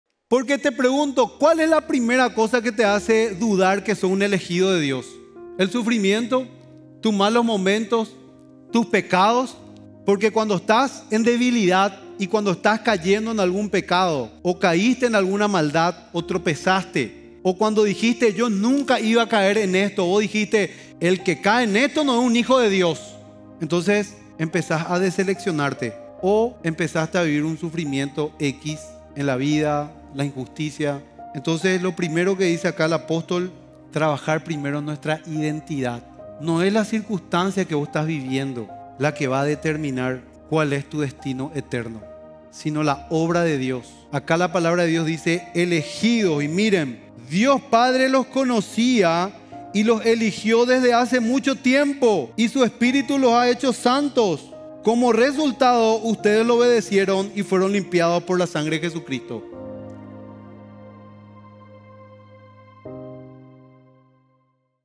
MDI 489 | 1° de Pedro - Prédica Pr Federico Almada.
¿Dudas de ser elegido por Dios? 🤔 No es la circunstancia del momento la que determina tu destino eterno, sino la obra de Dios en ti ✝️. Por su sangre fuimos limpiados, y ahora somos hijos de Dios 👑, sellados por Su amor y sacrificio ❤️.